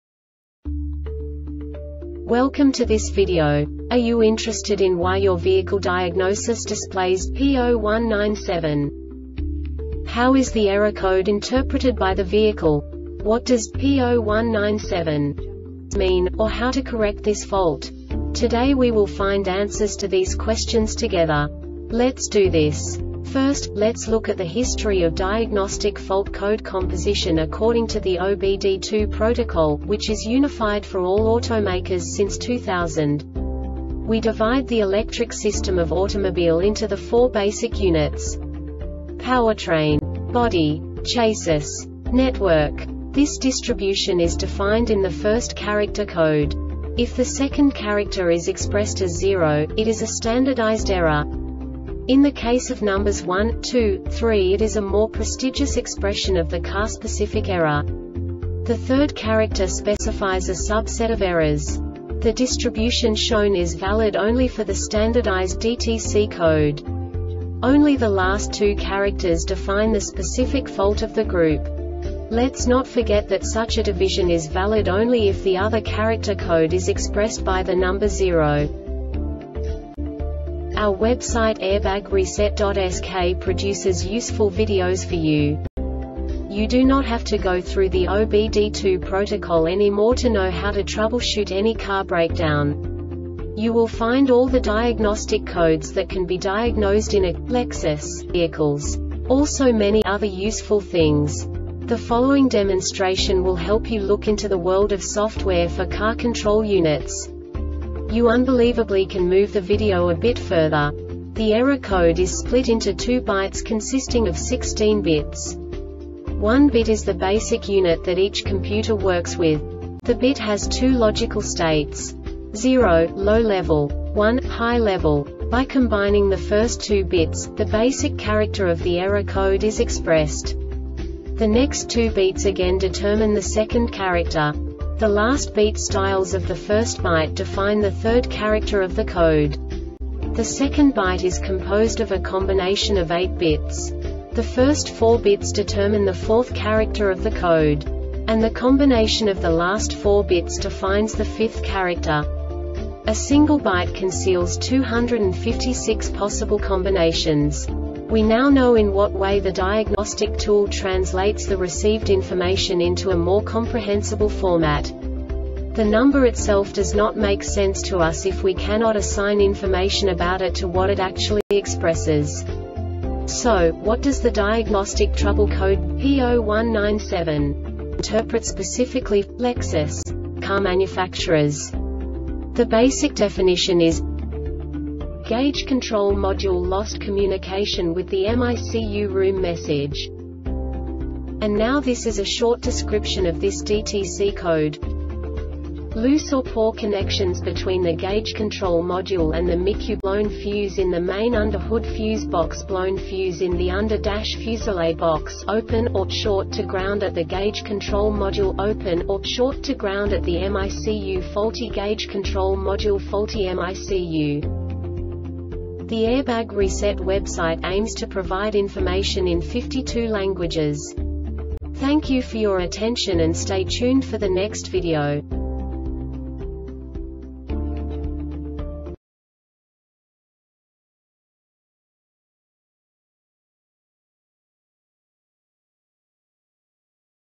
DTC Lexus P0197 Short Explanation
Contents:
0:21 Basic DTC analysis according to OBD2 protocol standard.
1:48 Insight into programming
2:58 A diagnostically understandable description of the Diagnostic Trouble Code
3:05 Basic error definition

Make:
Lexus
Code:
P0197
Definition:
Gauge Control Module Lost Communication with the MICU (RM Message)
Cause:
Loose or poor connections between the Gauge Control Module and the MICUBlown fuse in the main under-hood fuse boxBlown fuse in the under-dash fuse or relay box"Open" or "Short" to ground at the Gauge Control Module"Open" or "Short" to ground at the MICUFaulty Gauge Control ModuleFaulty MICU